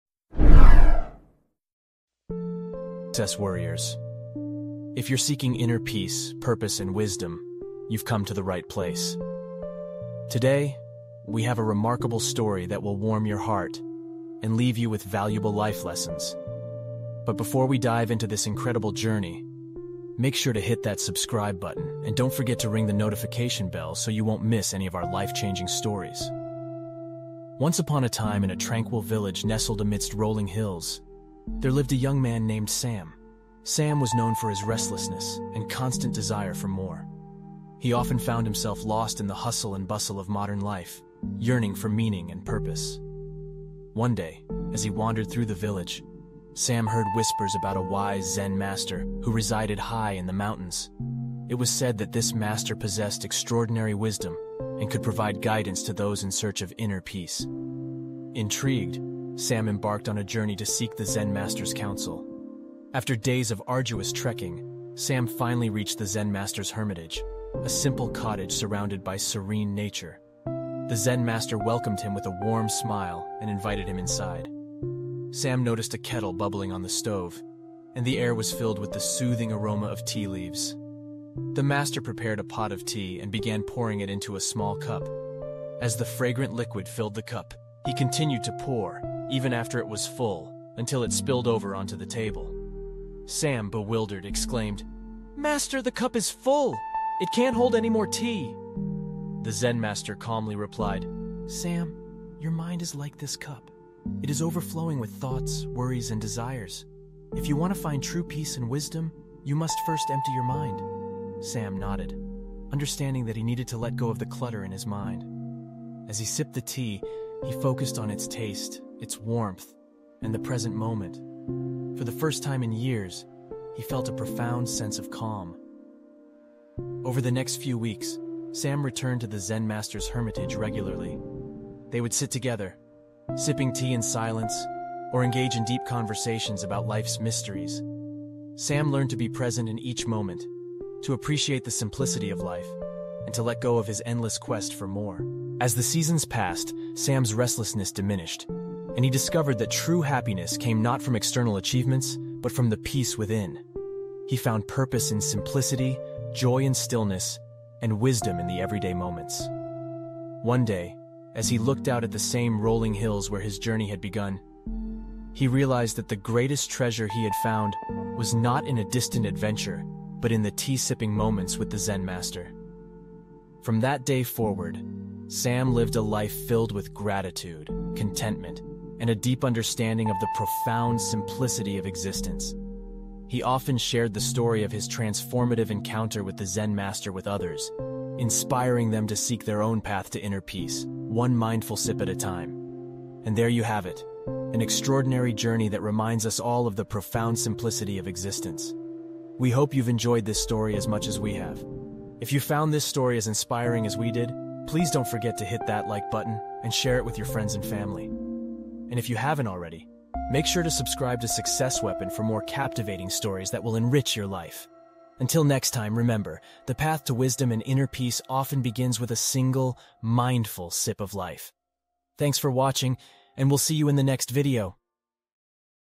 Transformative Tea Time with a Zen Master | Finding Inner Peace and Wisdom
Description:
In this inspiring and motivational story, join us on a journey of self-discovery and inner transformation. "Transformative Tea Time with a Zen Master" recounts the life-changing encounter between a restless young man and a wise Zen Master high in the mountains. Learn how the simple act of sipping tea can lead to profound insights and lasting happiness.

🌿 Discover the power of mindfulness and simplicity.
🌄 Find inner peace and purpose in everyday moments.
🧘‍♂️ Learn to empty your mind and embrace the present.